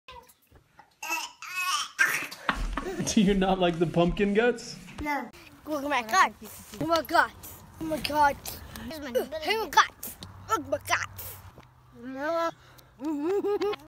Pumpkins make Wil gag!
Do your kids have gaging reflexes with certain textures? For years Wil used to gag if he barely touched the inside of a pumpkin. Carving pumpkins for him was like forcing someone with trypophobia to stare at honeycombs or tiny fruit seed patterns for 45 minutes. We’re not sure what changed because last night we had to keep telling him to stop putting pumpkin parts in his mouth. He was the gut miester and wouldn’t let anyone get the guts out of their own pumpkins (which worked out perfectly for the rest of us). Hopefully, this change means that he will be open to trying new foods besides potato salad all day every day.